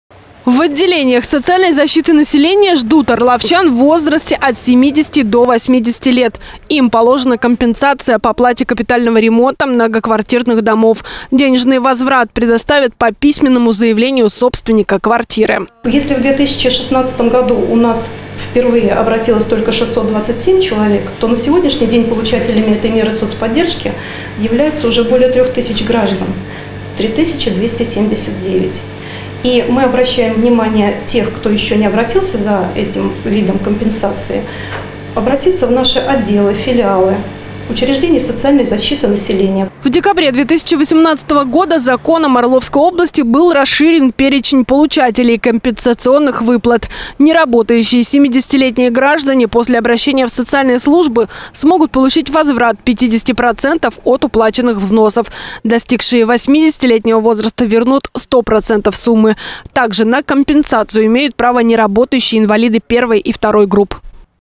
Пожилым орловчанам вернут деньги за услуги ЖКХ
Отдельным категориям пенсионеров возместят взносы на капремонт.